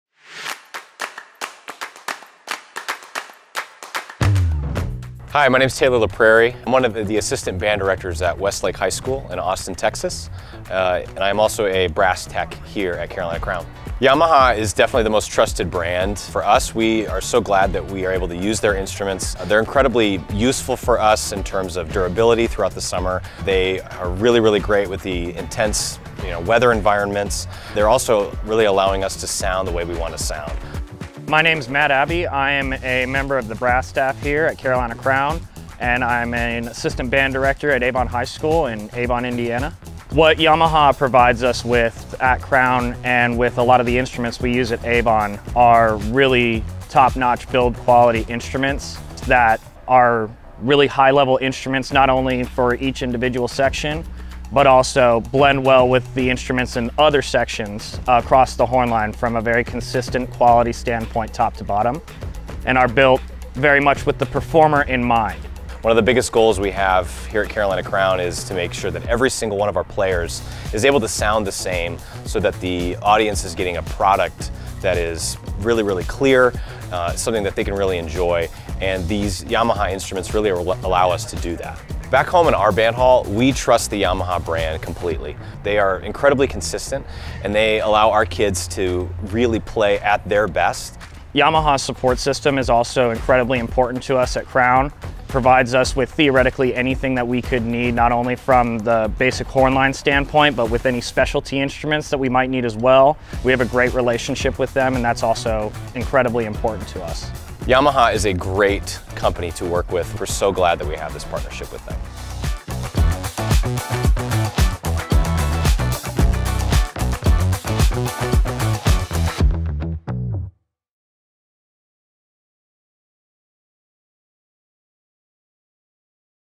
Yamaha | 2023 Crown Partner
The Carolina Crown brass and percussion sections choose to play the most world-class quality instruments provided by 𝗬𝗔𝗠𝗔𝗛𝗔 since 2007!

Crown Brass Alumni (now staff members) Matt Abbey & Taylor LaPrairie talk about their experience with 𝗬𝗔𝗠𝗔𝗛𝗔 instruments both at Crown and at their own programs!